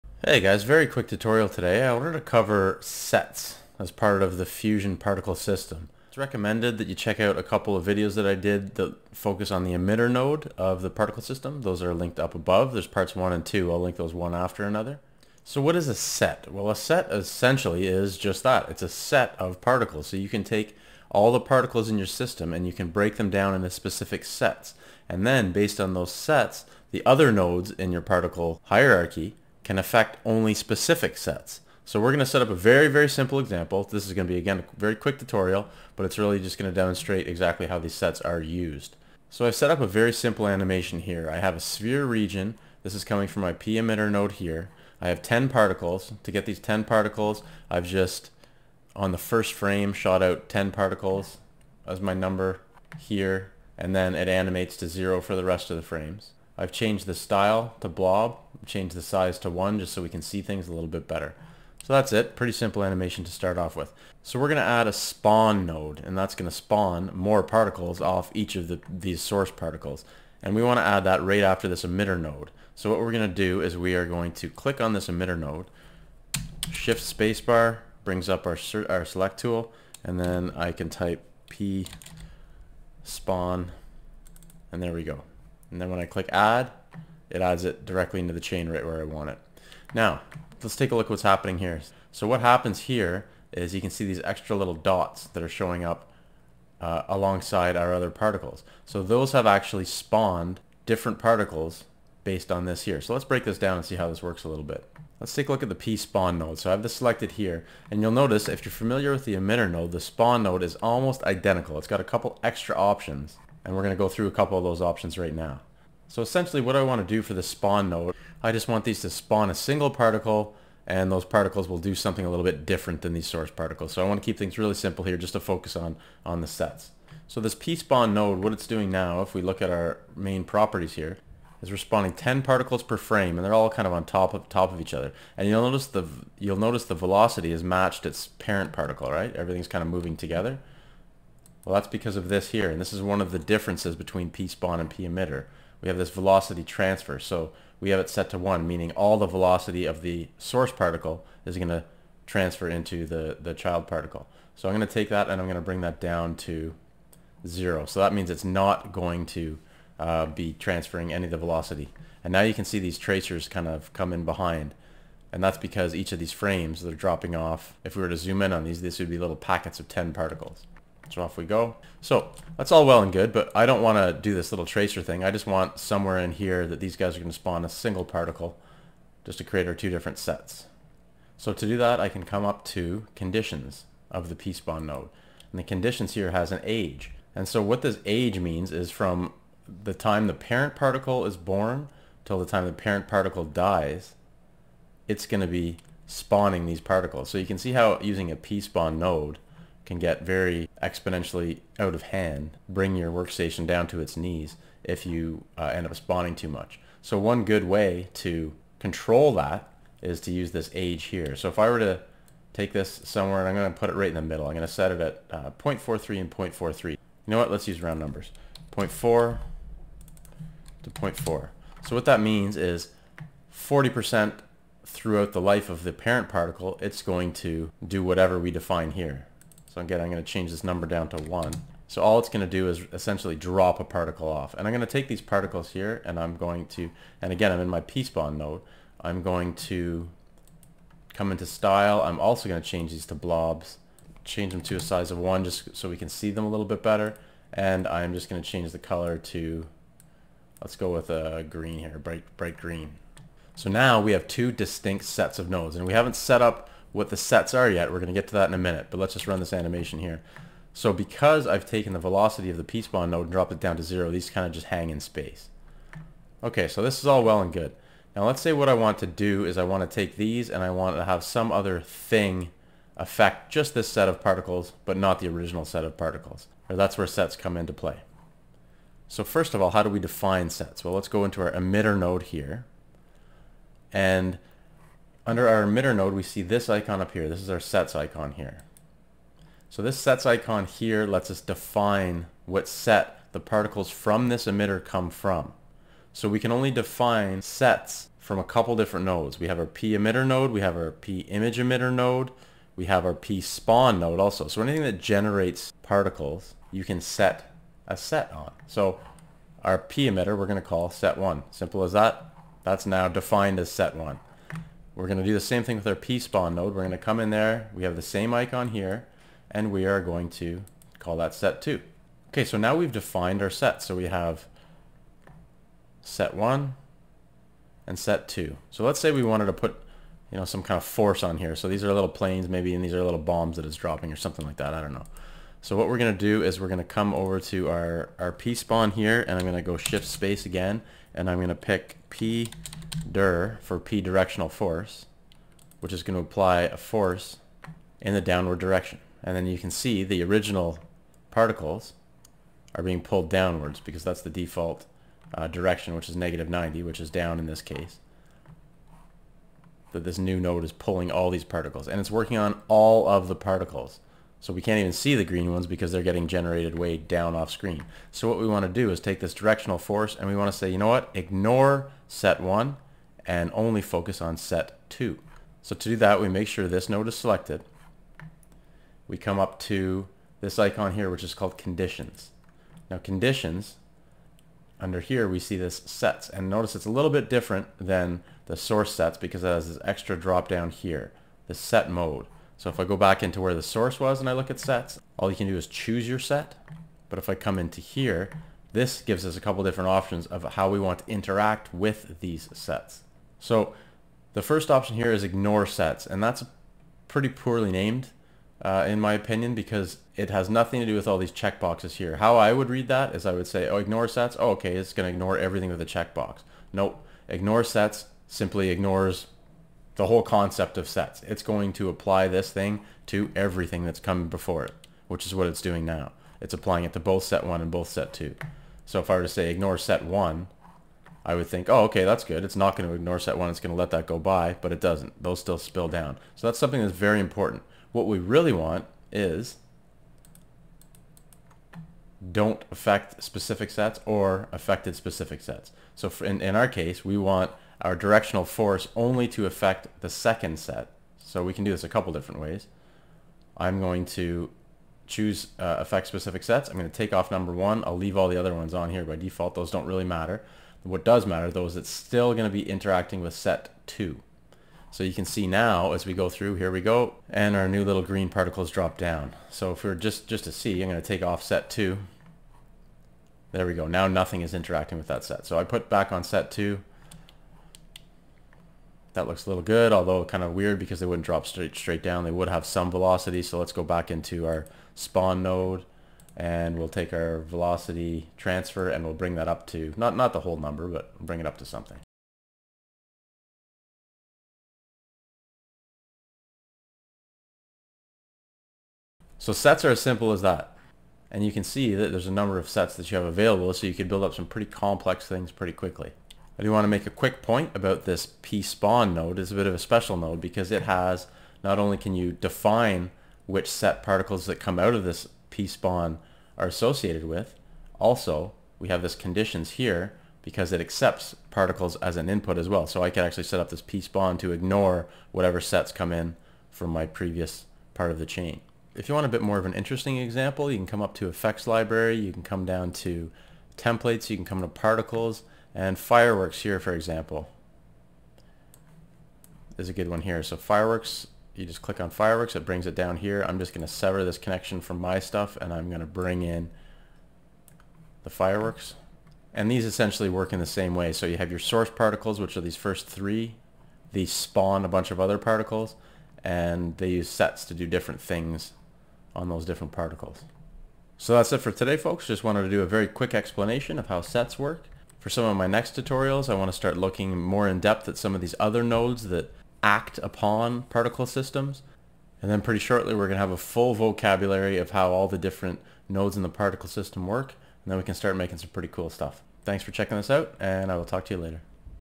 DaVinci Resolve 16: Particle Sets Tutorial
A brief look at how particle sets work in DaVinci Resolve Fusion 16.